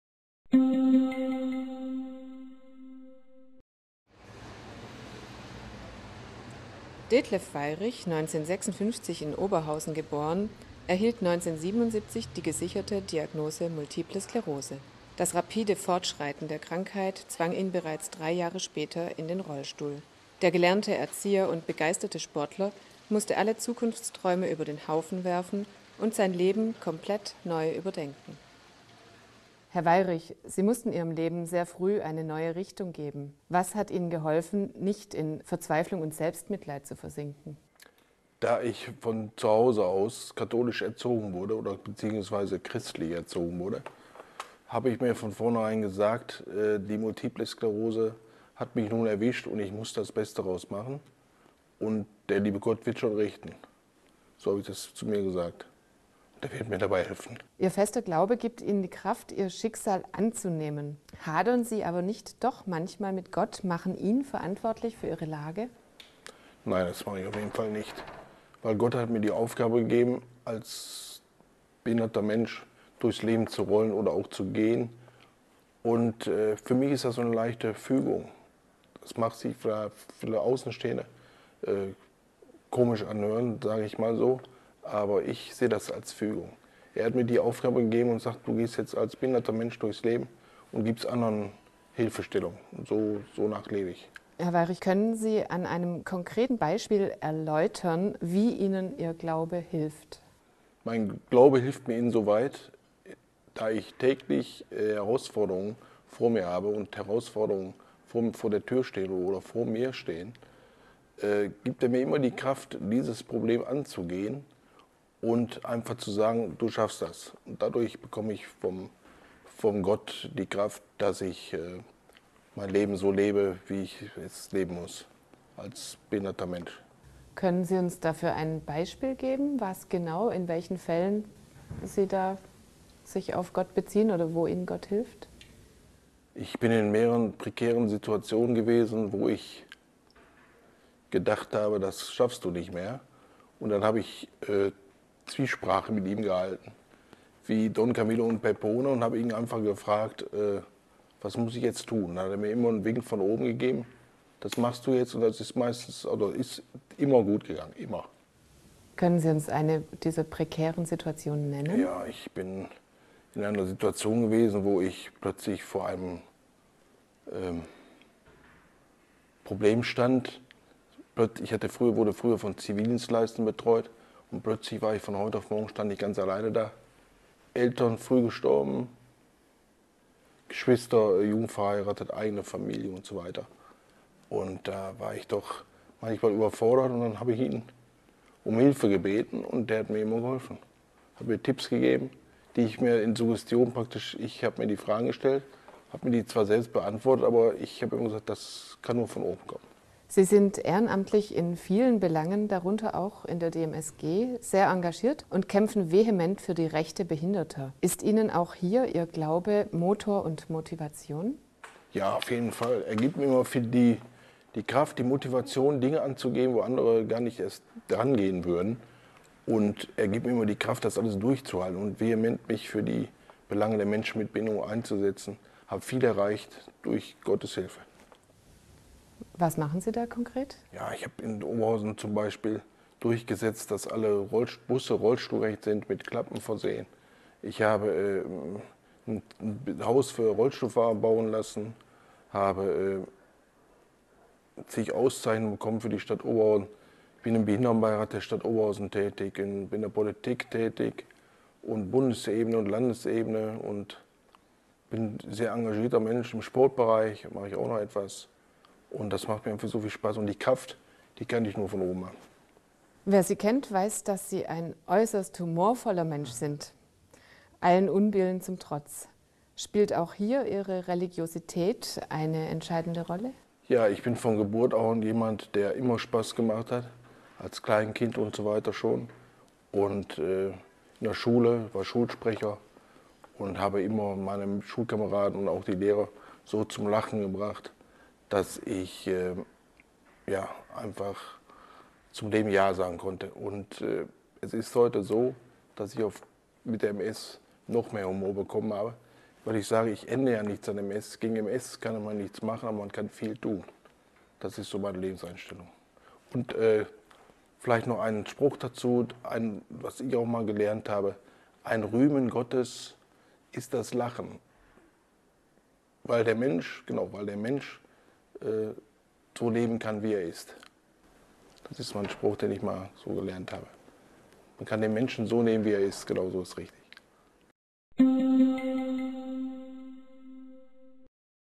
Glaube und Krankheit (Multiple Sklerose)
Detlef Weirich erhielt mit Anfang 20 die Diagnose Multiple Sklerose und saß kurze Zeit später bereits im Rollstuhl. Der Glaube an Gott hilft ihm, sein Leben zu meistern und sich für andere zu engagieren.